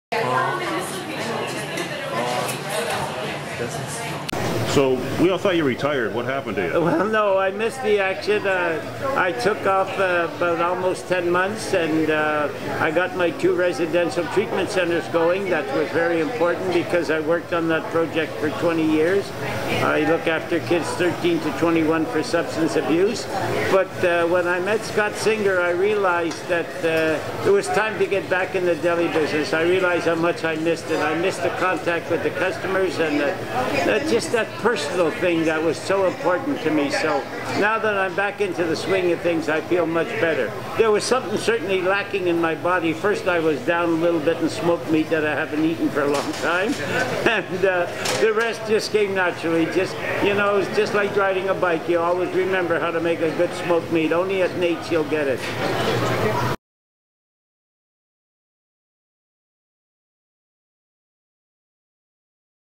Egan column: Nate's Deli
Dave Smith has partnered up with Scott Singer to re open Nate's Deli, this time on Merivale Rd.